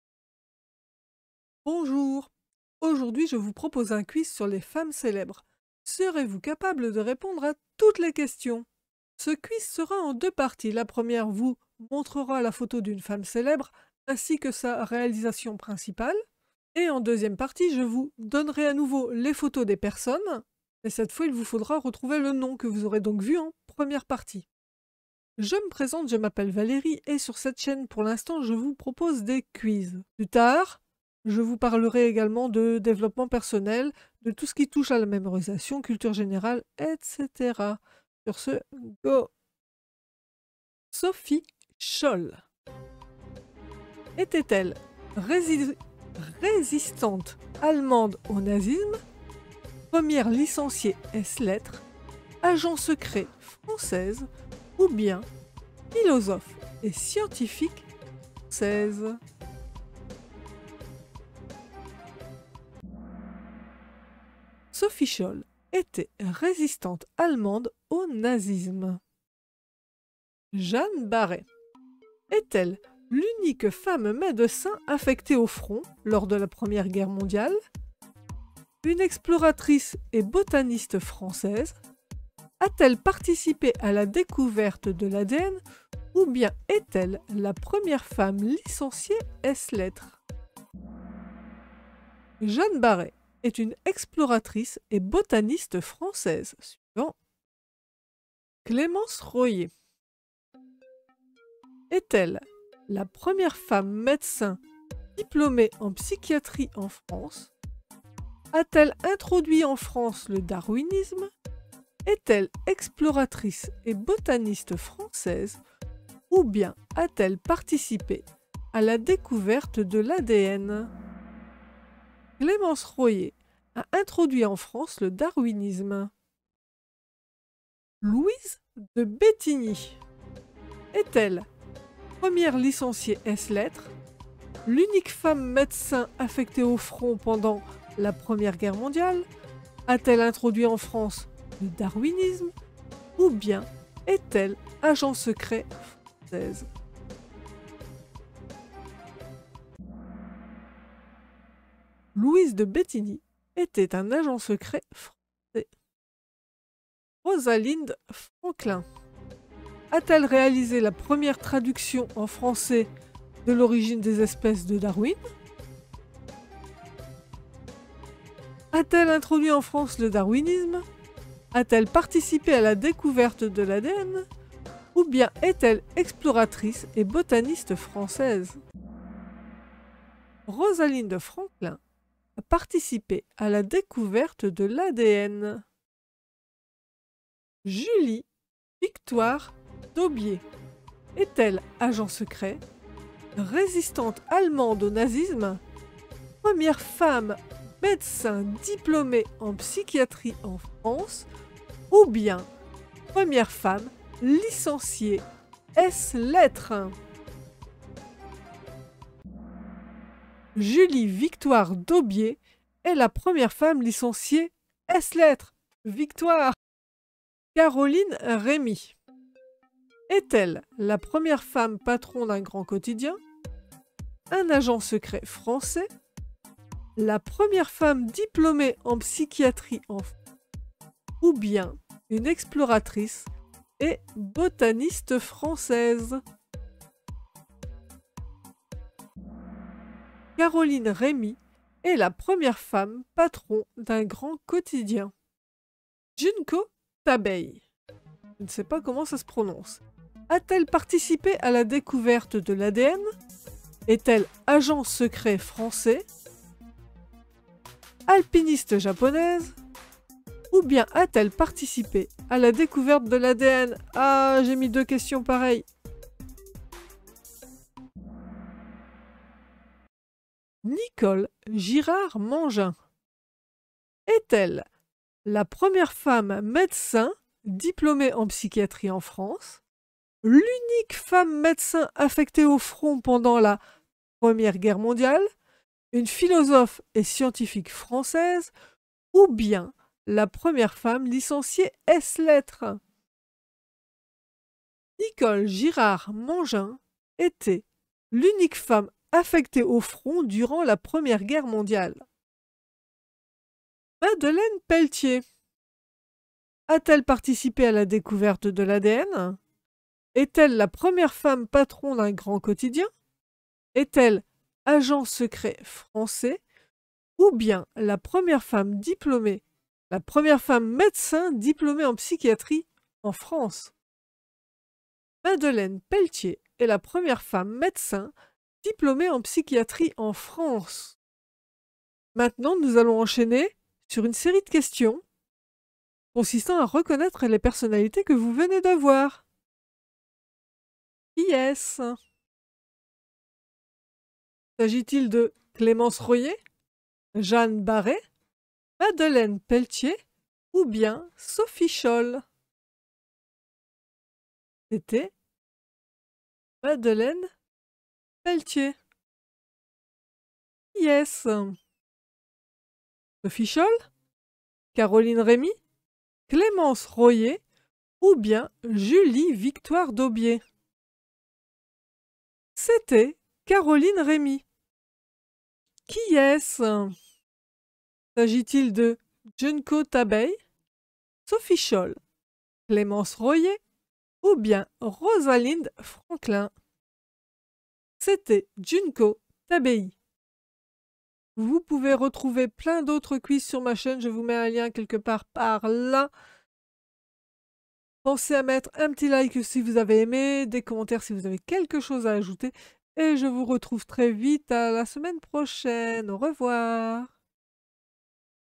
1er quizz série femmes célèbres ; les connaissez-vous ?
vous voulez améliorer vos connaissances ? Ou votre mémoire des noms et des visages ?
Ce quizz est fait pour vous ! Le mieux étant de le visionner plusieurs fois.🙂
Une pour voir les réponses ; une pour voir si vous avez retenu les réponses ; et encore d'autres fois pour ancrer la mémorisation.

J’ai fait, pour ce quizz, deux séries :

Dans la première vous pourrez trouver le nom des femmes, une photo ainsi que leur réalisation principale.

Dans la seconde, je vous montre à nouveau les photos des personnalités vues précédemment, et je vous donne 4 propositions de réponses pour trouver quel est leur nom.

Vous pouvez aussi, bien sûr, partager votre savoir dans les commentaires sur la vidéo.

Dites-moi, c'est comment pour vous : trop facile, bien dosé, trop dur ?
Combien de réponses avez-vous trouvé ?

Quizz réalisé à l’aide de kahoot.com
Images Wikipédia, sauf information contraire (ou erreur de ma part)

Si vous avez aimé ou si vous voulez m'aider : pensez au pouce en l'air et aux commentaires sous la vidéo. 🙂
Merci